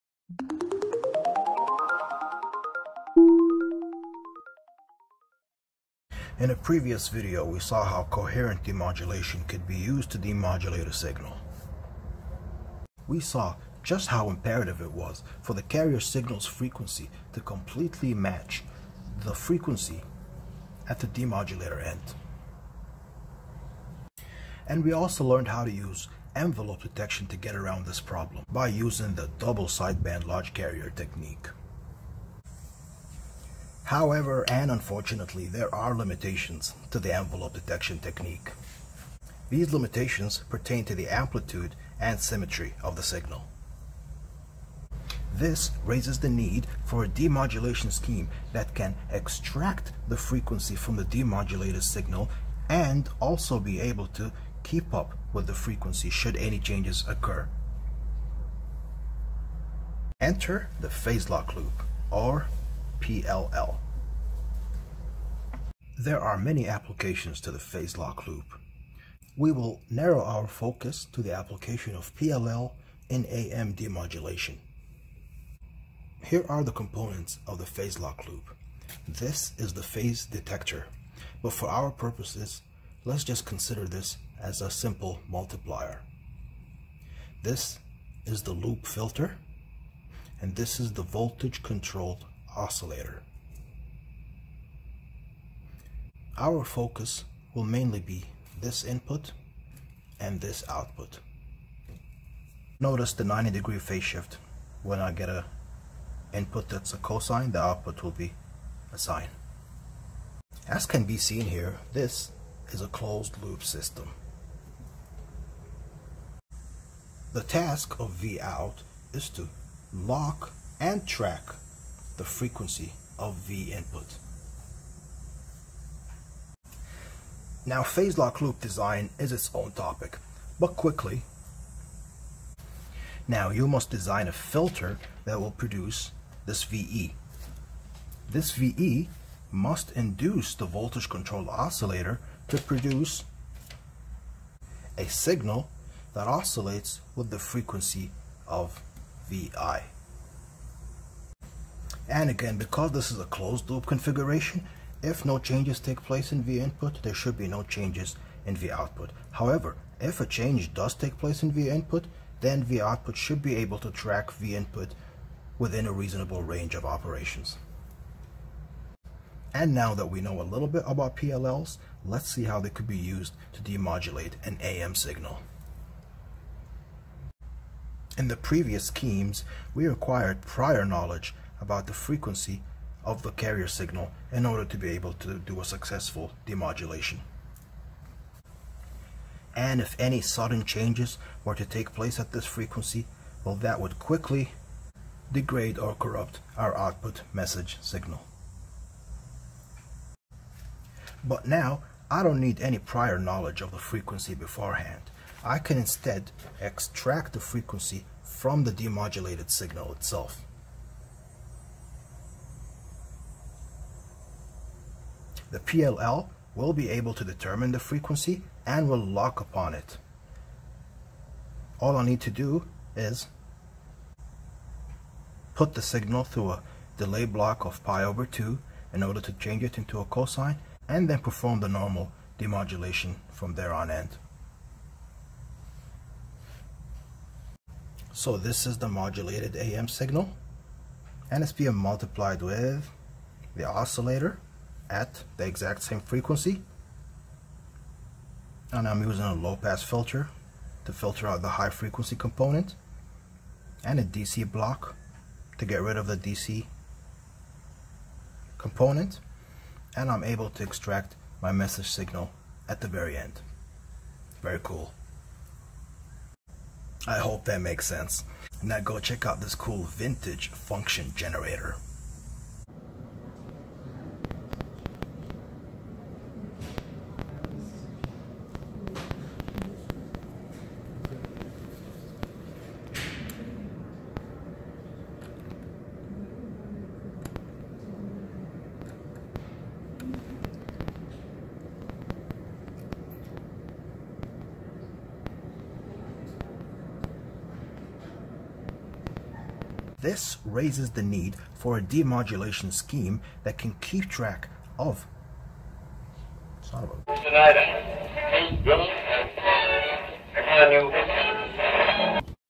Part 2: AM Demodulation with PLL (Phase Lock Loop)
Here in part 2: I show the basics of how the Phase Lock Loop can be used to demodulate an AM signal.
Remember to watch part one for greater understanding.